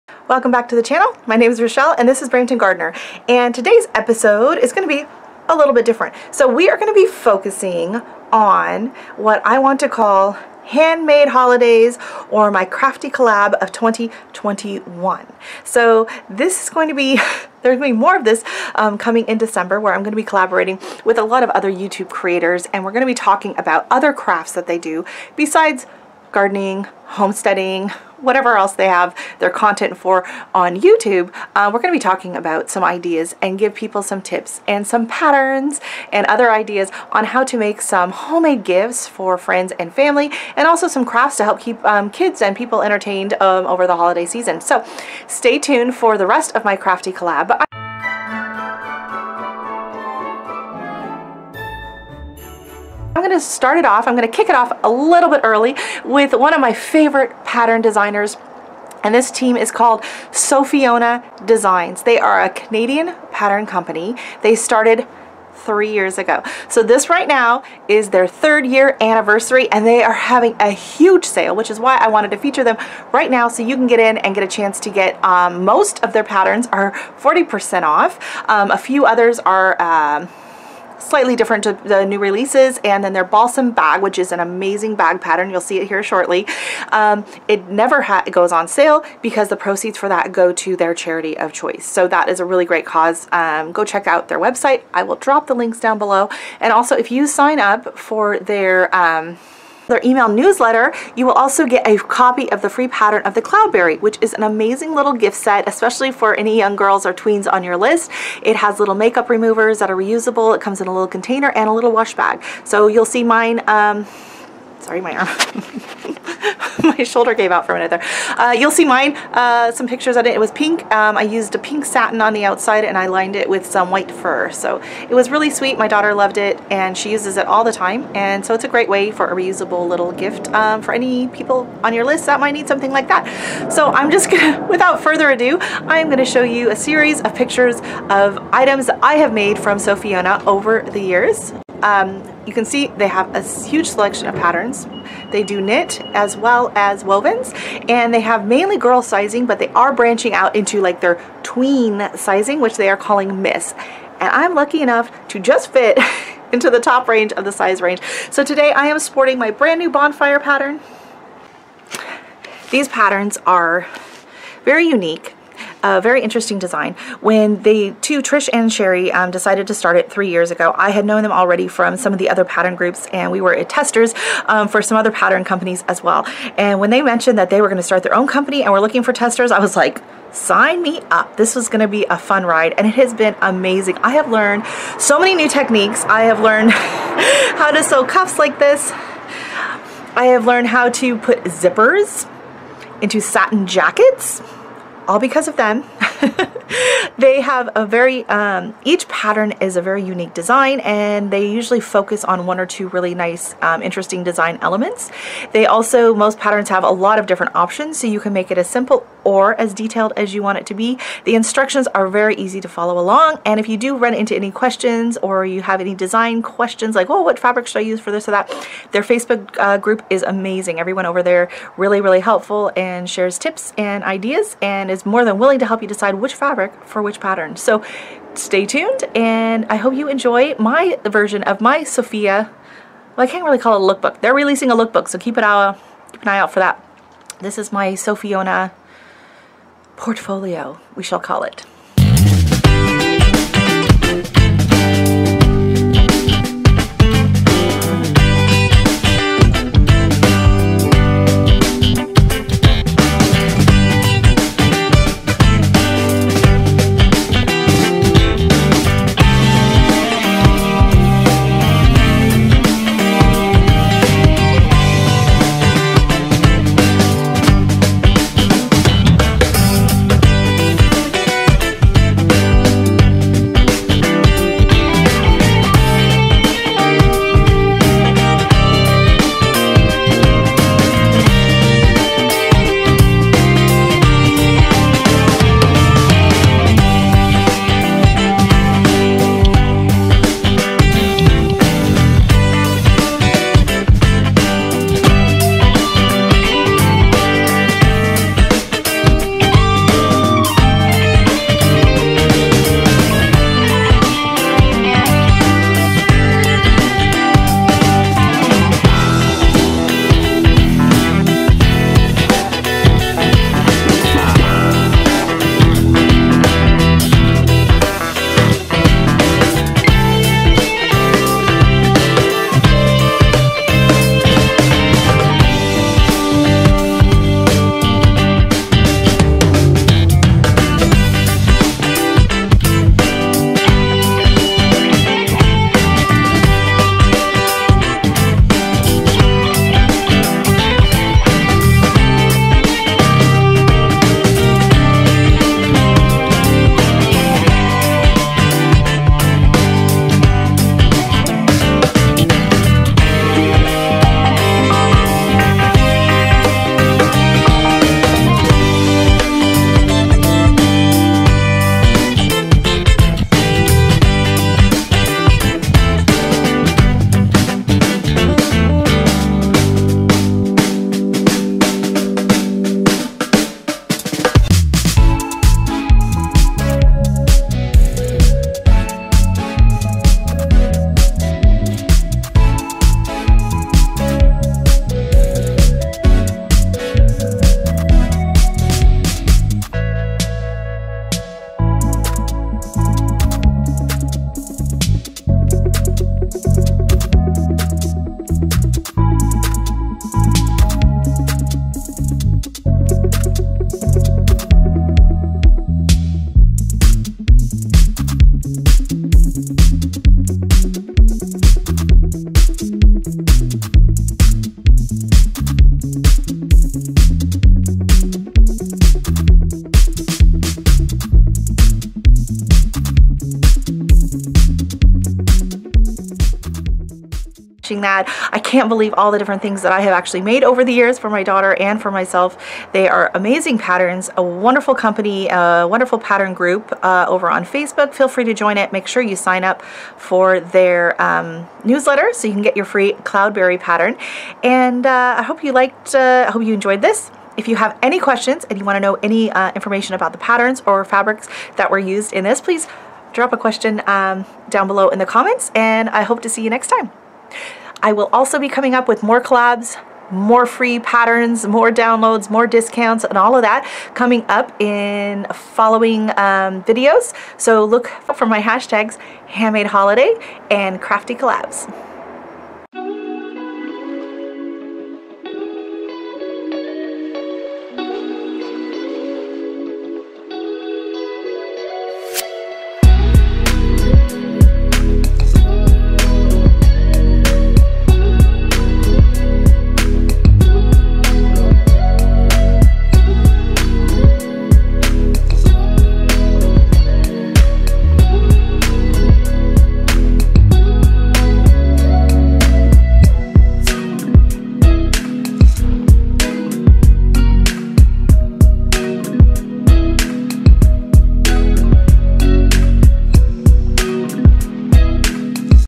Crafty Holidays! Ep 1 Sofiona Designs 3rd Anniversary!!!! 40% off Sale
Sofiona Designs 3rd Anniversary! 40% off Sale

Trying to become more self-reliant, self-sufficient on your homestead??? Wanting to learn to sew???  Lots of patterns will be linked, for free or great sales.  If you sew or have a friend who sews be sure to share this link with them.

Sofiona Designs is a great Canadian Pattern company. 40% off Nov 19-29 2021
Here is the link to their awesome new Look Book

Sofiona has some Amazing Free patterns, Cloudberry (newsletter)

The pinecone skirt has a free code when you join their FB group, link is in the pattern description.

Need help with your channel?  Sign up for TubeBuddy with my affiliate
Gardening in a suburban Garden in Canada in zone 5b/6a.